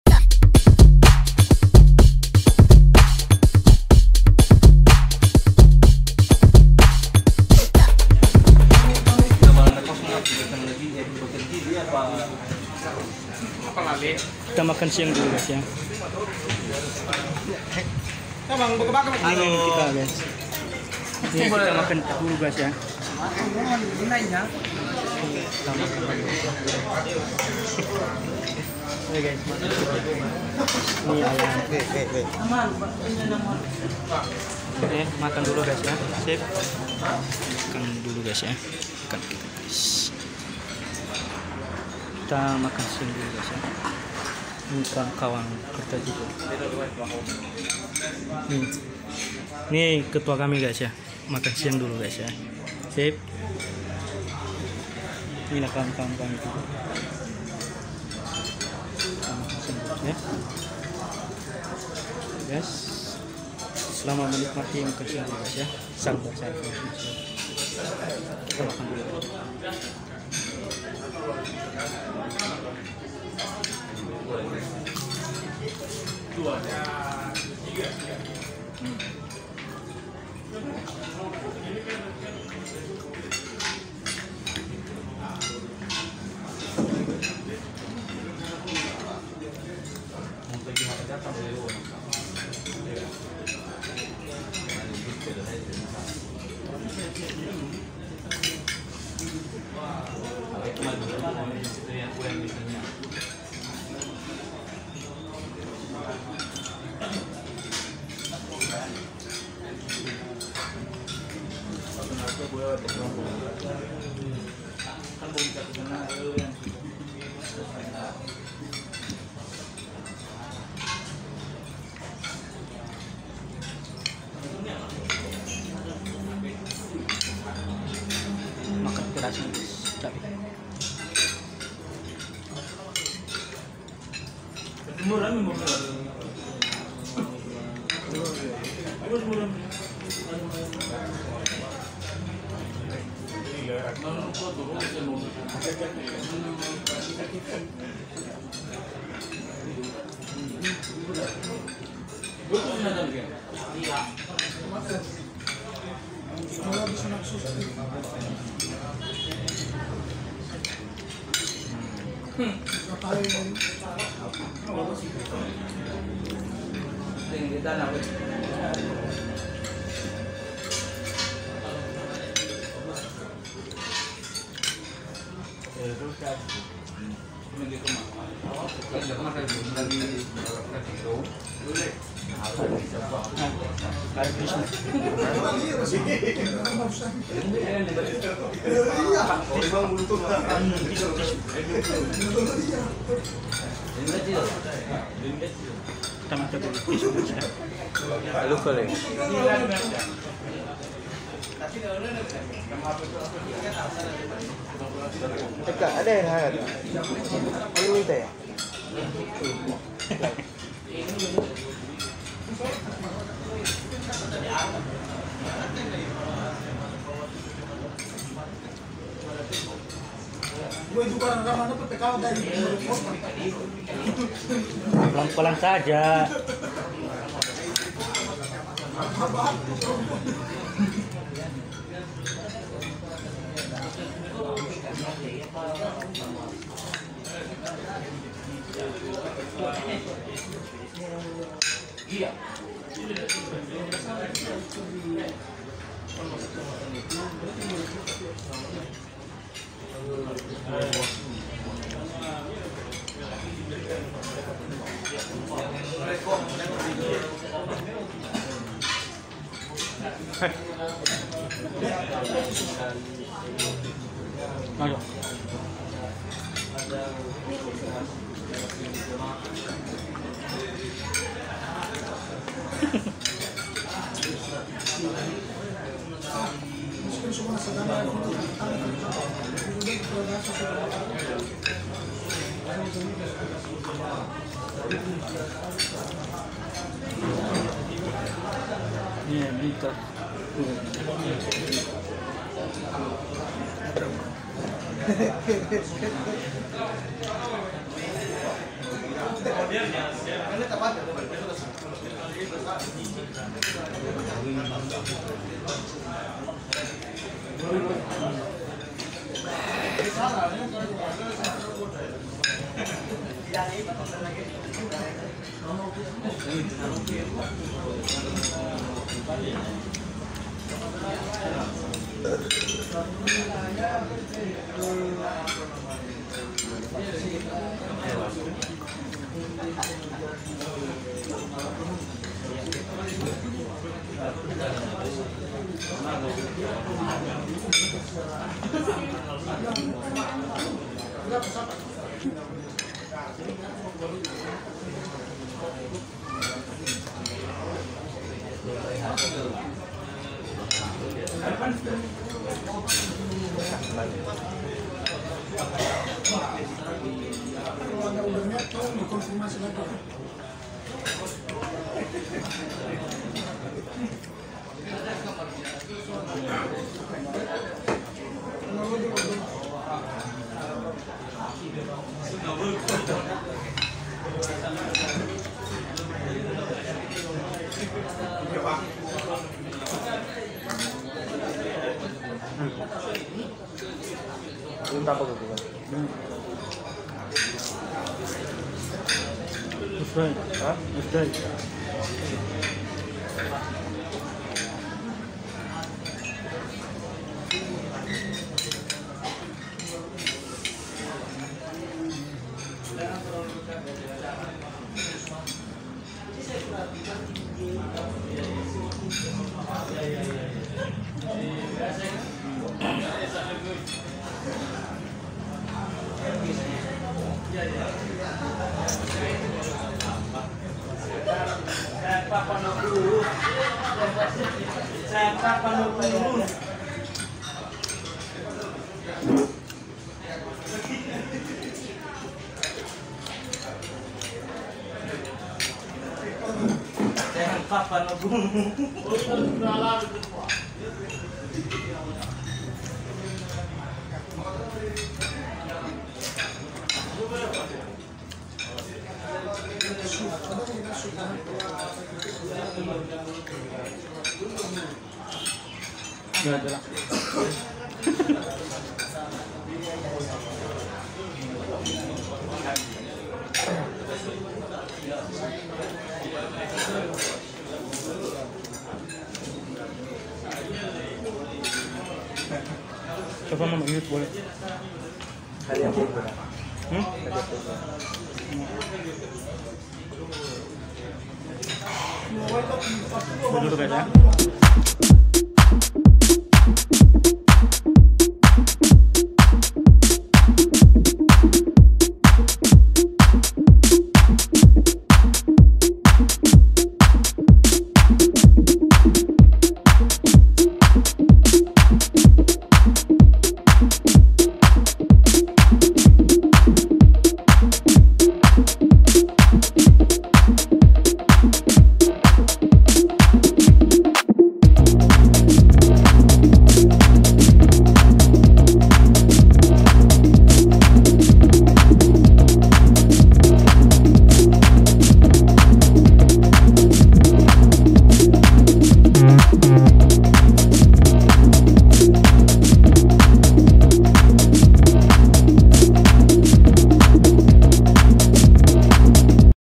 Review Makan di Restoran Hotel Soliga
hotel soliga nias
hotel soliga gunungsitoli
hotel wisma soliga

 Makan siang di Hotel Soliga  Pulau Nias
 Jl. Pangeran Diponegoro, Miga, Nias Sumatera Utara

Salam dari saya
Fayruz Nias Channel
Ama Lovis